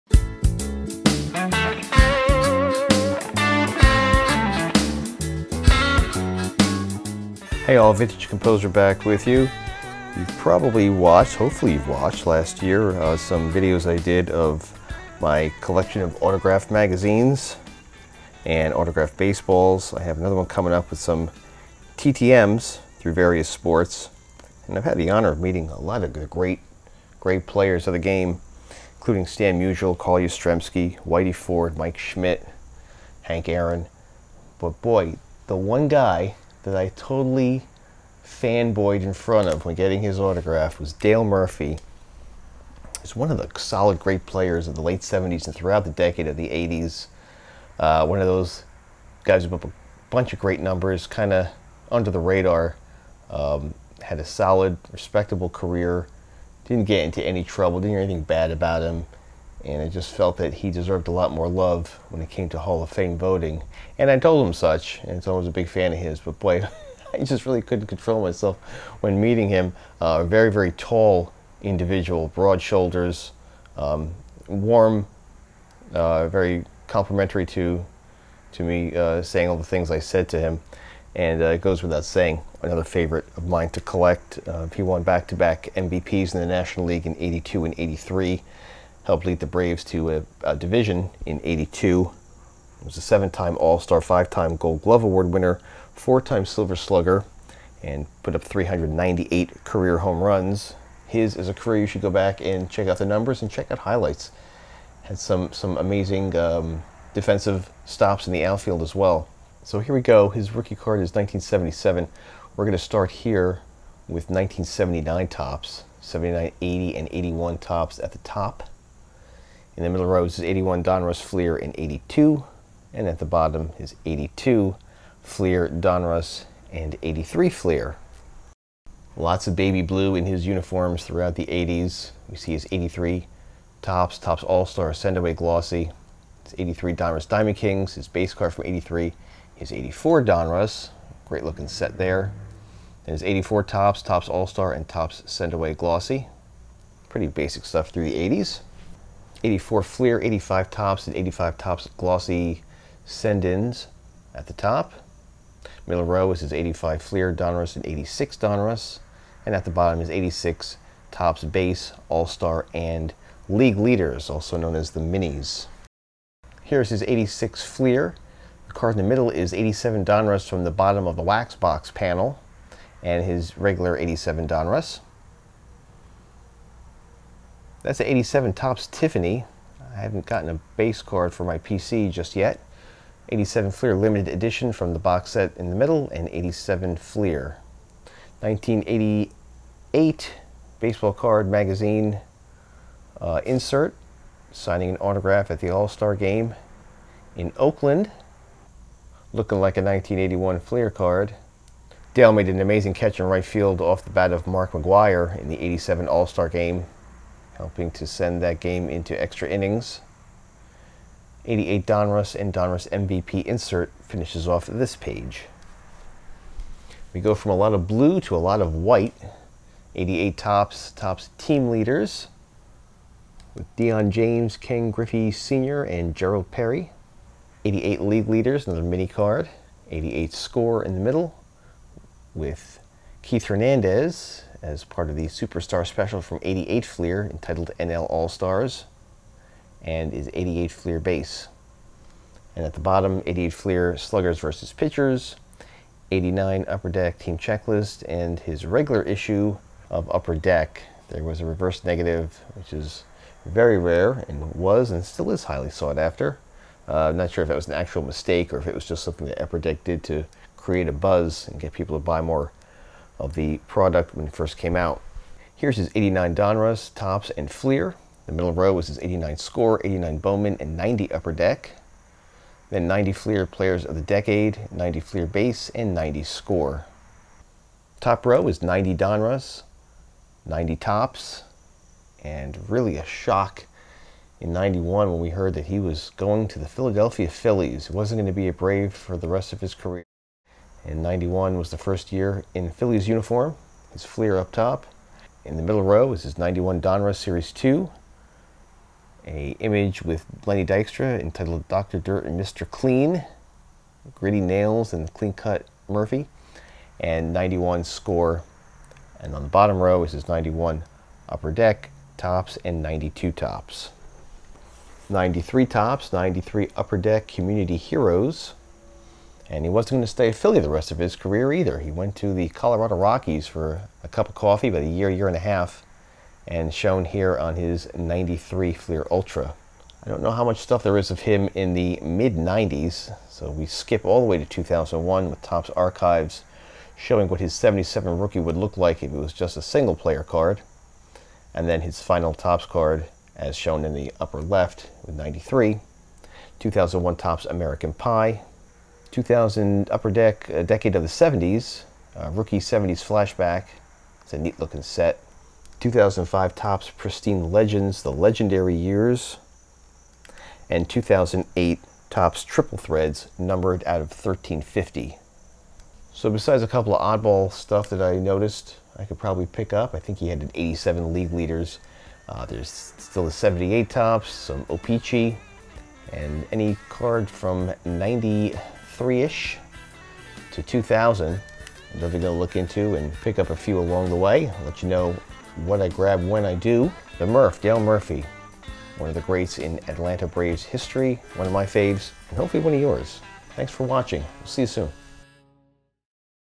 My Dale Murphy PC • The Vintage Composer
A key figure in the 1980’s baseball scene, here’s a Brave look at some cards of the 2x National League MVP.